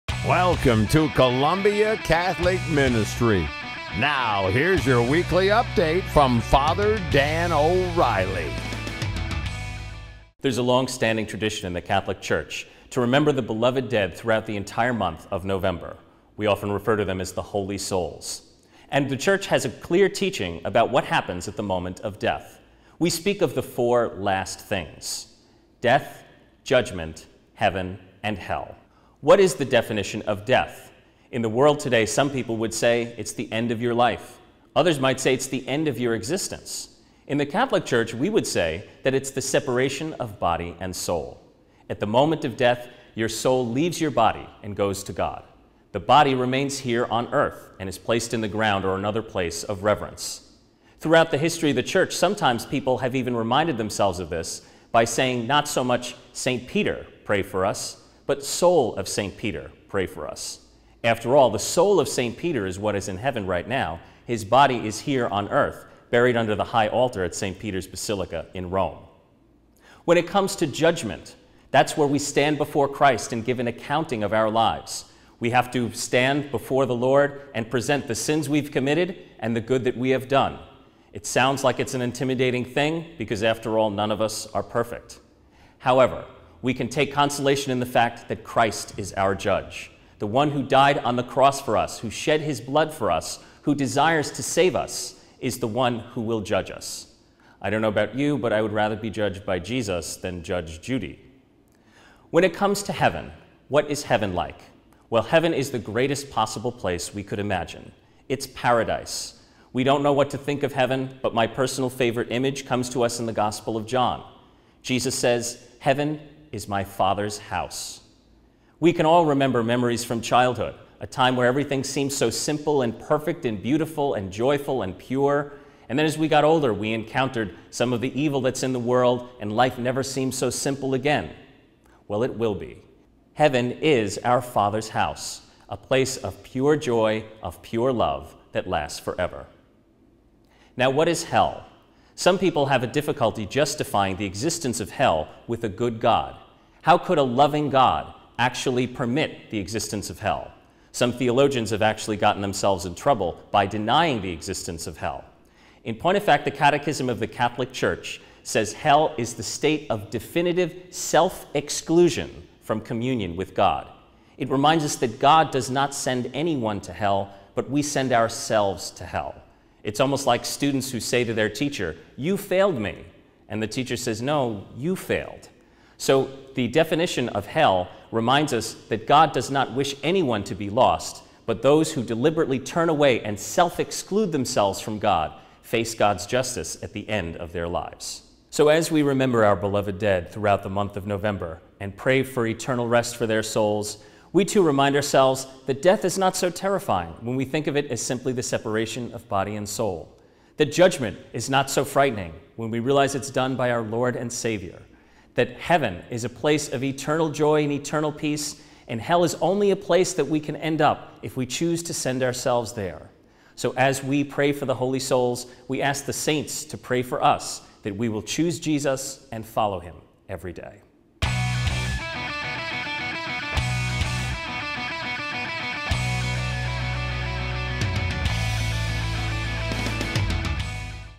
The Four Last Things | Columbia Catholic Ministry
What is the definition of death? Father Dan O'Reilly discusses church teachings on what happens at the moment of death.